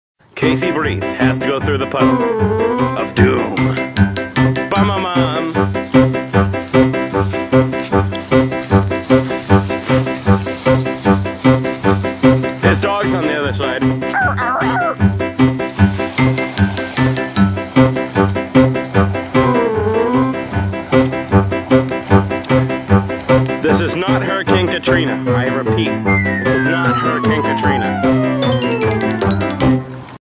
casey the clown
casey riding his bike through the puddle of doom